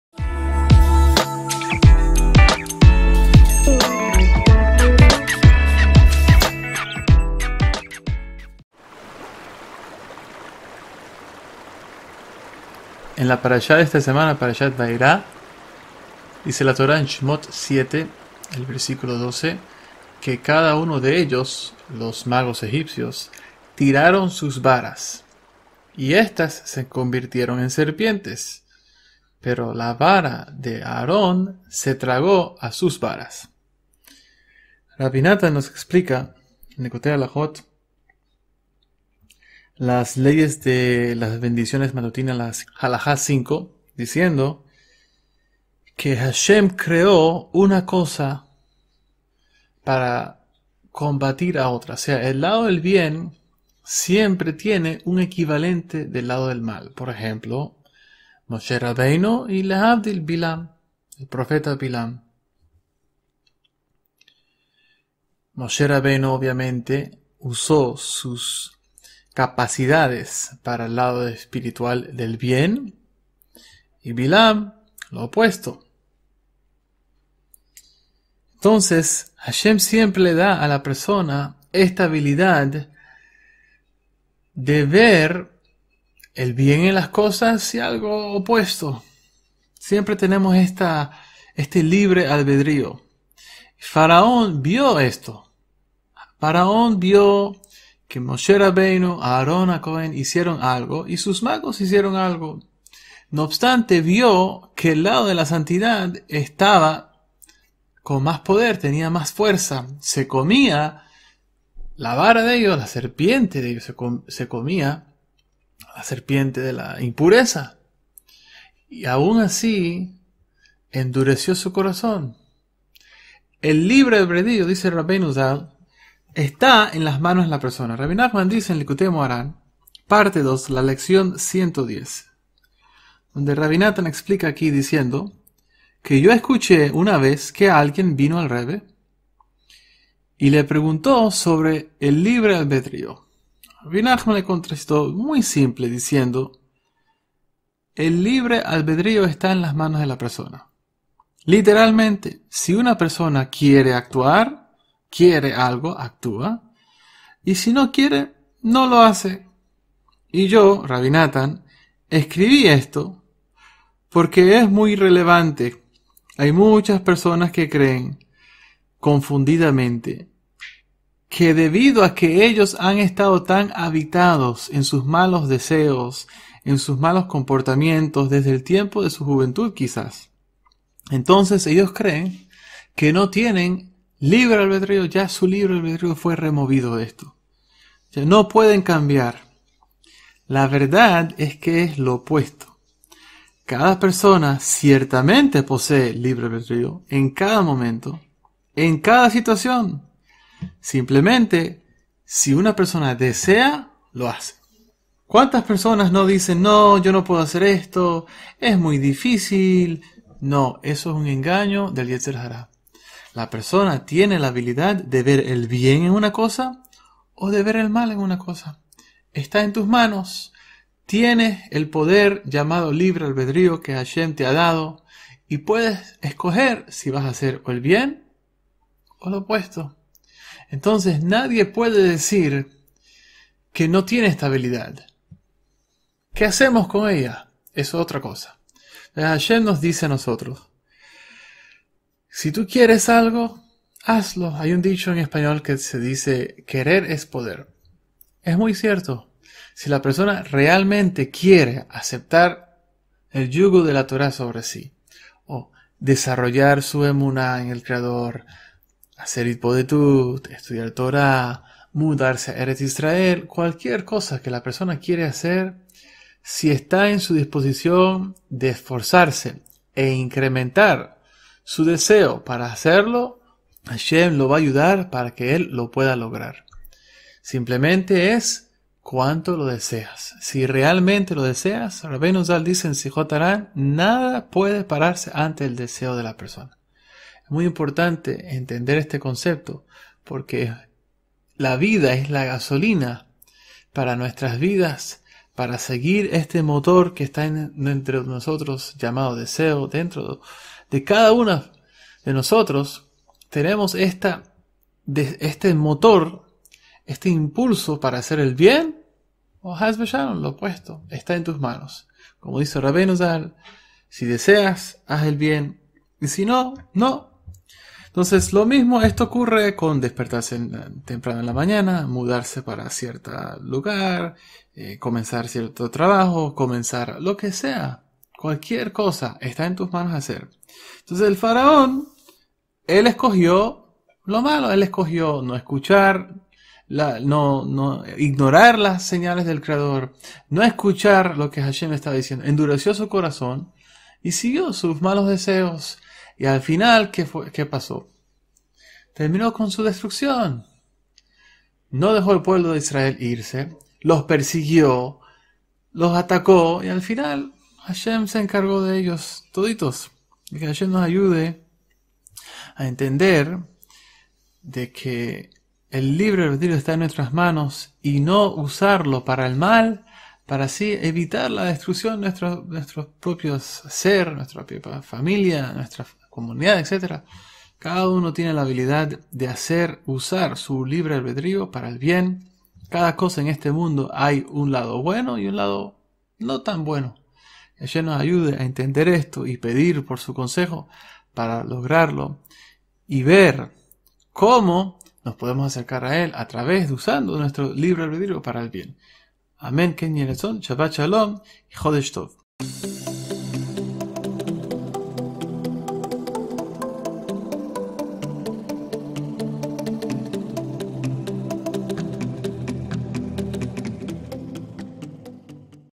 Parashát Va'erá 5777
Hashem creó el mundo con un balance, donde todo lo que hay en el lado de santidad existe en el lado de la impureza. ¿Por qué? Esto es para darnos libre albedrio para escoger entre el bien y lo opuesto. ¿Cómo podemos usar el poder del libre albedrio para acercarnos al Creador? Veamos... con el Rabino Natan Hayyim.